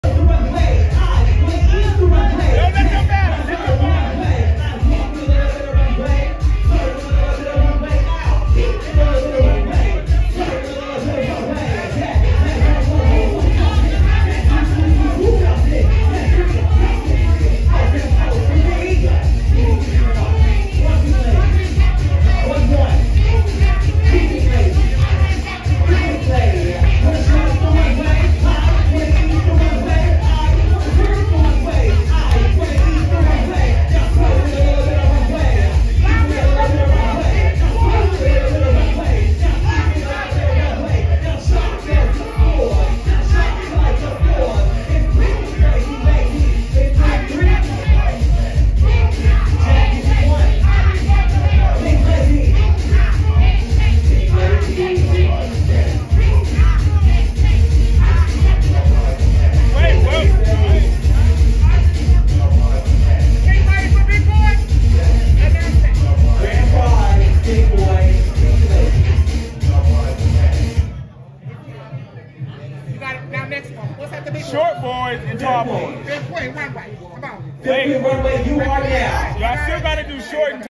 OTA RUNWAY: BIG BOYS | MKE MOTHER BODACIOUS HENNESSEY & BALLROOM THERAPY | STARS WARS KIKI BALL
MKE MOTHER BODACIOUS HENNESSEY & BALLROOM THERAPY PRESENT:
"WARS OF THE STARS": STARS WARS KIKI BALL

MC: KUSH GOT BARS
DJ: LEGENDARY FOUNDING MOTHER INDIRAH Ragé

MILWAUKEE OTY APPROVED!

MIDWEST OTY APPROVED!